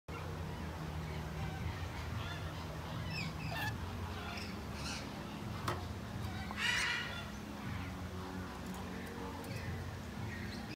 MONTE CASINO BIRD GARDENS, 2019
Great place to go in Johannesburg. Really nice! Travel Tip from the PAPOUSCI – the czech parrot magazine,